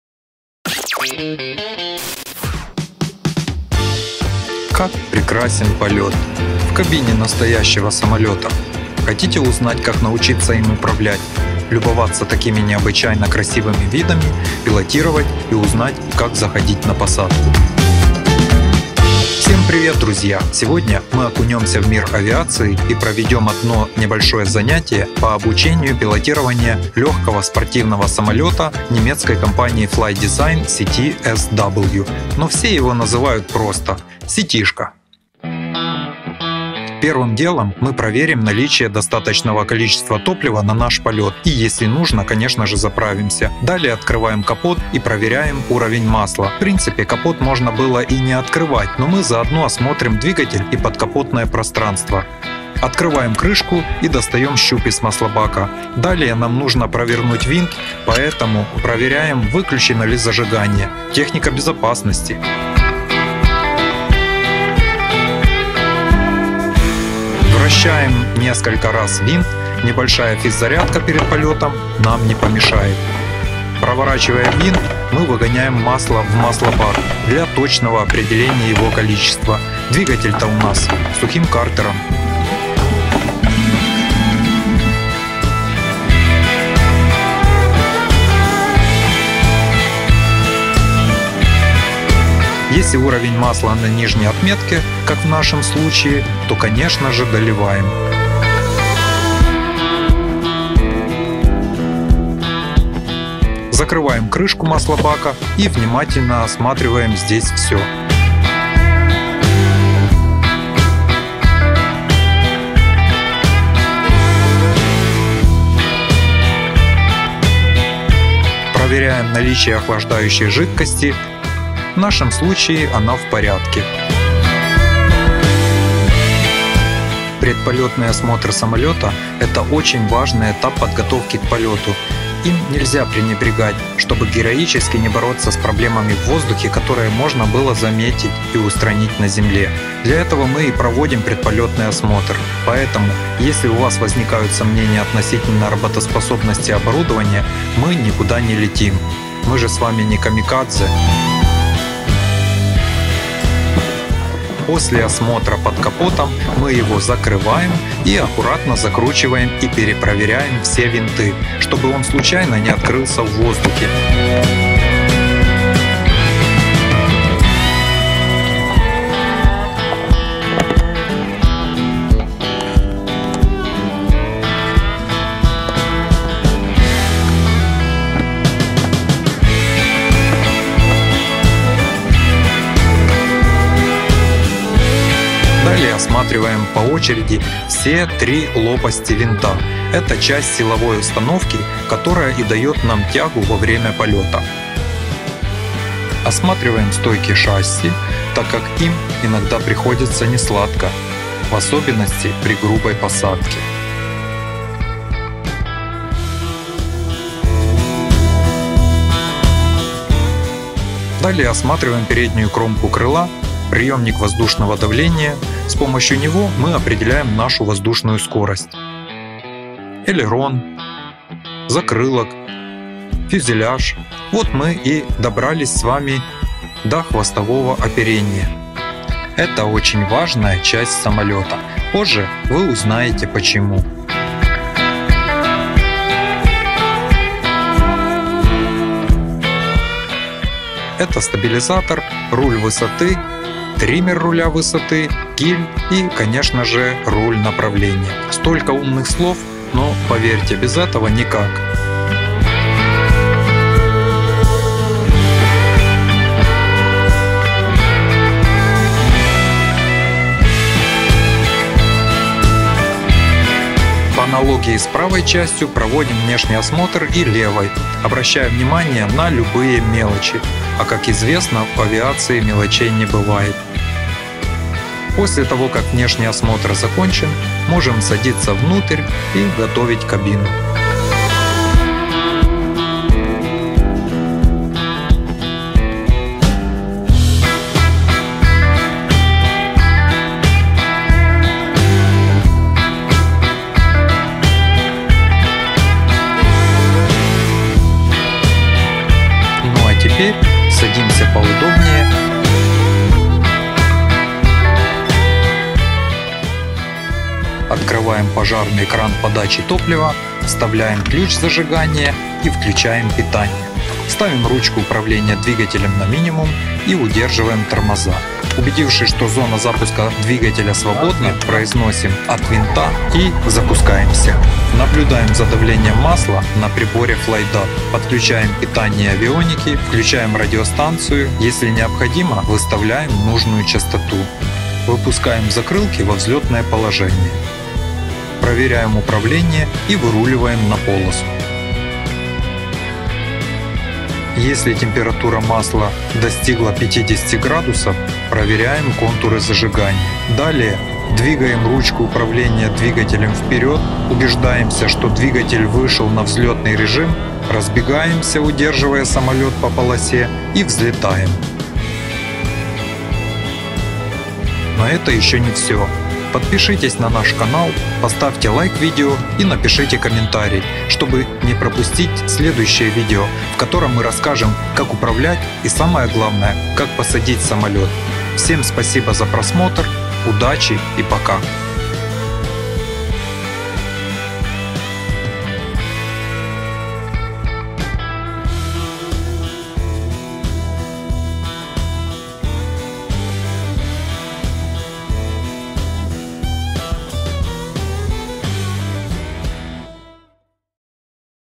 Малая авиация. Полёт на самолёте. Как научиться летать на самолёте.
Авиация общего назначения или малая авиация. Полёт на самолёте CT-SW. Как научиться летать на Ситишке  от Flight Design.  Двухместный частный самолёт, обучение пилотированию. Это видео об этом и об авиации в целом. Легкая авиация и сверхлегкая авиация - учимся летать!
0:00 Полёт на частном самолёте
1:07 Предполётный осмотр самолёта
5:19 Запуск двигателя
5:51 Взлёт самолета
В этом видео вы узнаете как научиться летать на самолете ctls, ctsw.👍🛩 Мы затронем темы:
малая авиация обучение, предполётный осмотр, аэродинамика самолета и самолётовождение так как это всё важные части обучения пилота! Расскажем кто такой частный пилот и как получить PPL лицензия пилота.